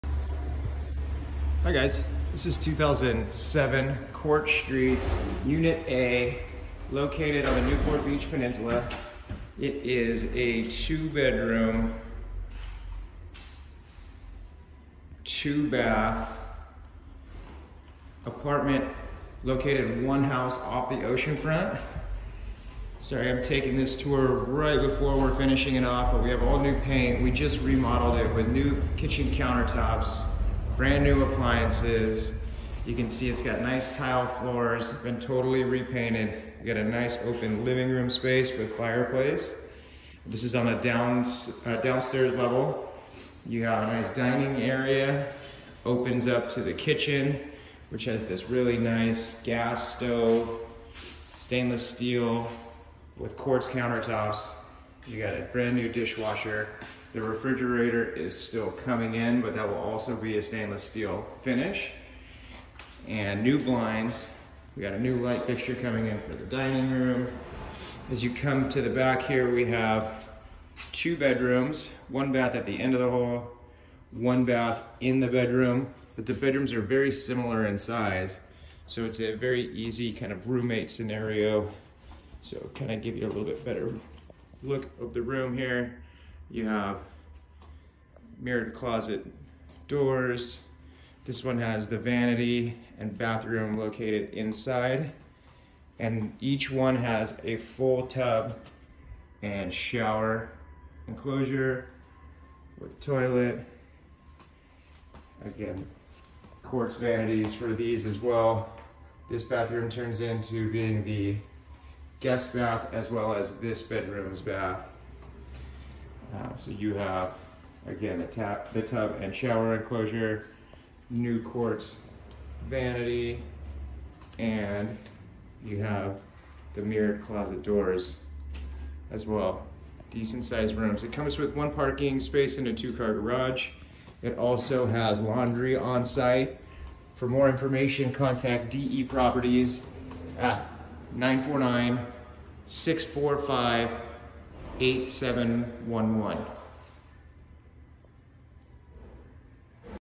2007 Court St
Totally renovated 2 bedroom 2 bath located on the Newport Beach Peninsula just one home from the sand.